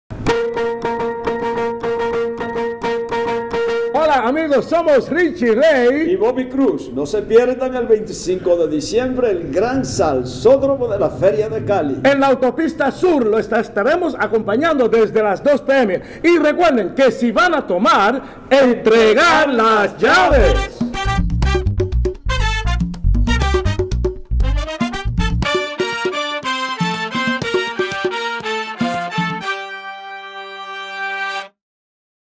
Richie Ray & Bobby Cruz invitan a disfrutar del salsódromo
Richie Ray & Bobby Cruz invitan a disfrutar este 25 de diciembre de 2013, el Desfile del Salsódromo de la 56 Feria de Cali.